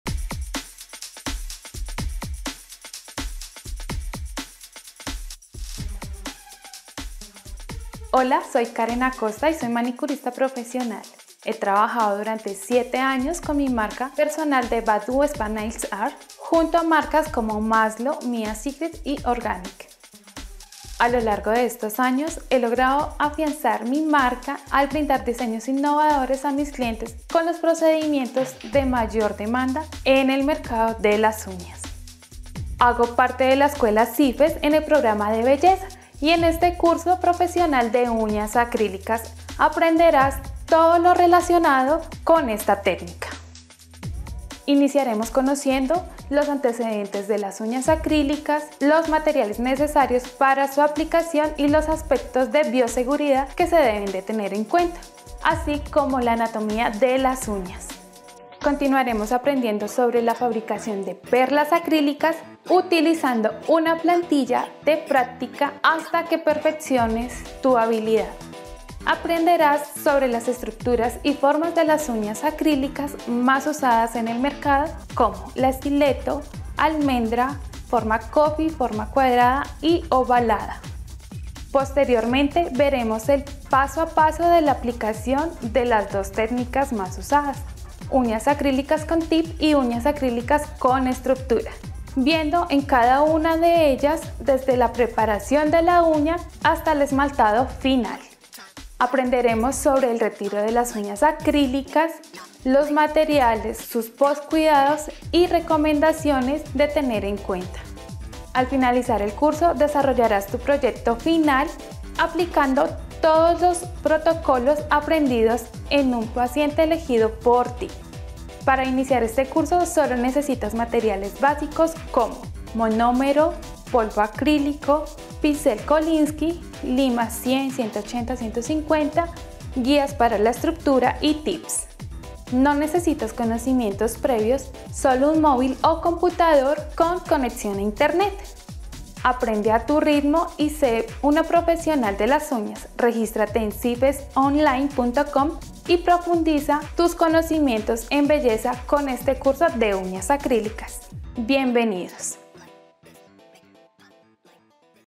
Curso de Uñas Acrílicas aprende el paso a paso para realizar un procedimiento profesional
. En este curso podrás aprender paso a paso como colocar uñas acrílicas, comenzando por los diseños más básicos, luego pasando a conocer el protocolo para la aplicación de formas y estructuras y por último el protocolo de uñas acrílicas con tip.

Además, también conocerás las recomendaciones post aplicación para mejorar la durabilidad de las uñas acrílicas. En el transcurso del curso realizarás prácticas, por lo que al culminarlo contarás con los conocimientos y destrezas para una correcta aplicación de uñas acrílicas.

💙 Escríbeme al WhatsApp para mas información de los cursos - Haz